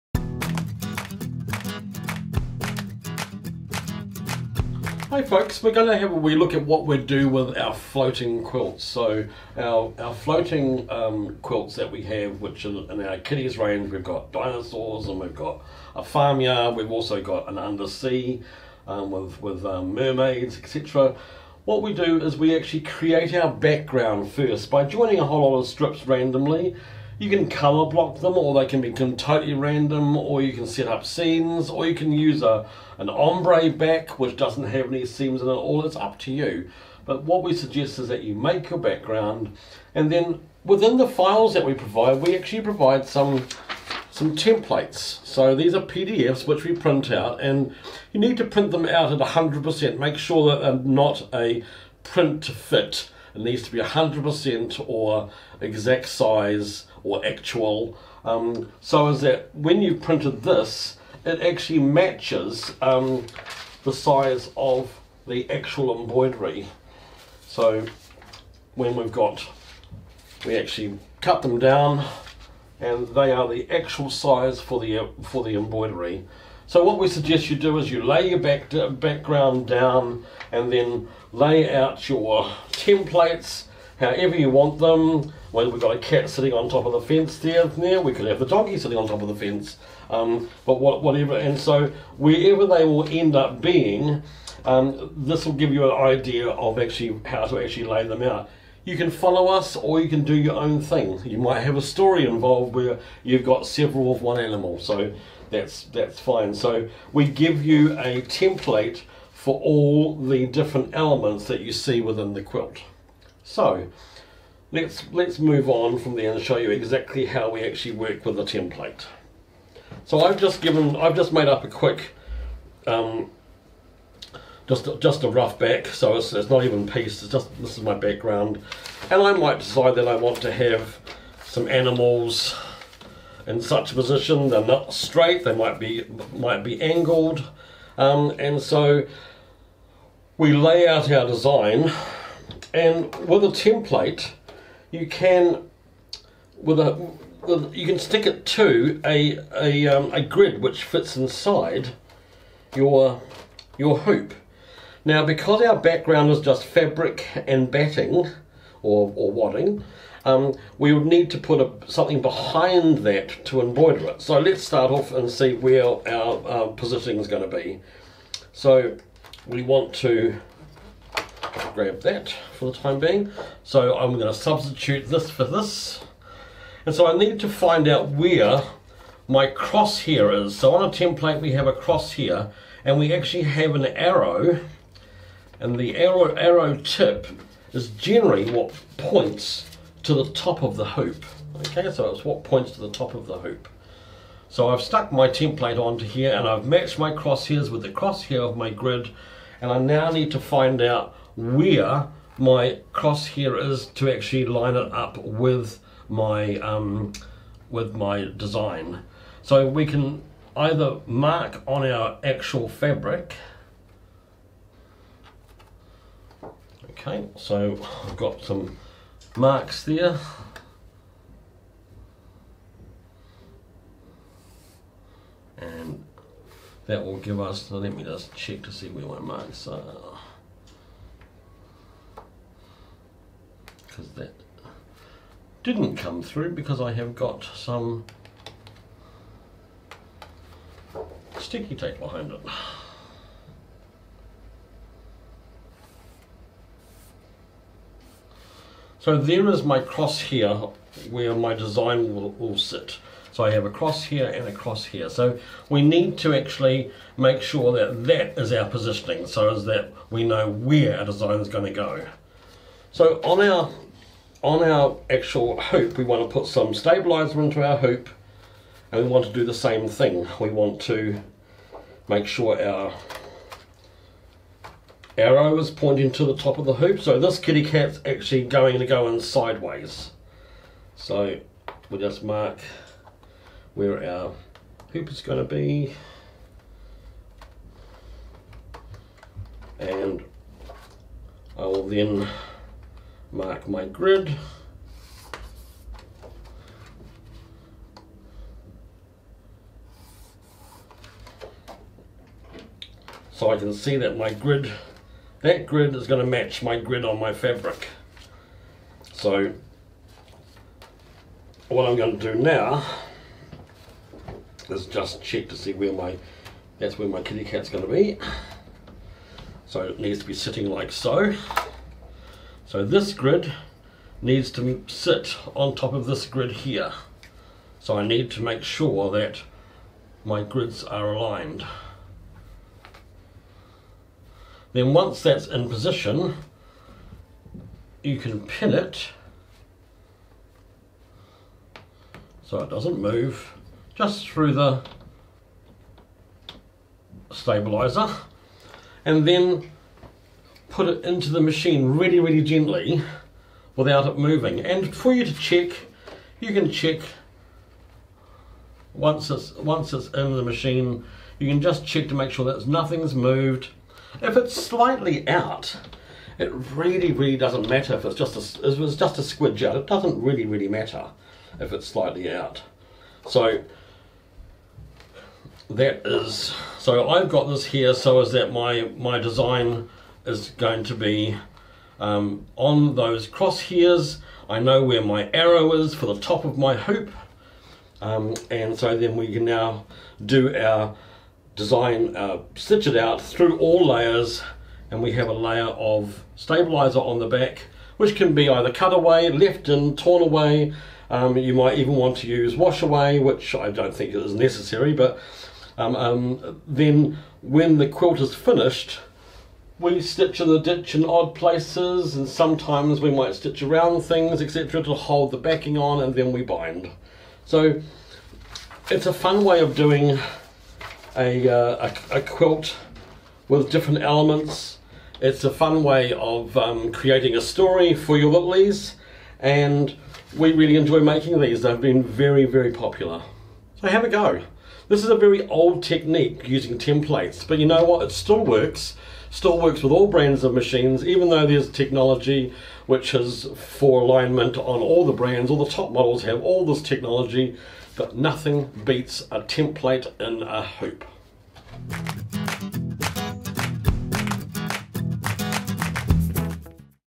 How to Make Floating Quilts - With Martyn Smith
We are having a 50% sale on all our Floating quilts from the 21st - the 24th April, 2022

We are going to have a look at how to make a Floating Quilt.
A floating quilt is a quilted or single panel fabric background with appliqué and embroidery stitched over the top.

The best part about a floating quilt is you can make the background and size of the quilt, any way you like.
You can create your background by joining a whole lot of strips, like a jellyroll quilt.
This can be done randomly, colour blocked or set up as a scene.
You can use a single panel of fabric like an ombre or a patterned fabric to make the background for your quilt.
Your imagination is the limit.

Once you have your background picked out or stitched together you then get to floating your in the hoop applique and embroidery around the quilt.

We have a number of amazing options for you to choose from if you would like to give a floating quilt a try: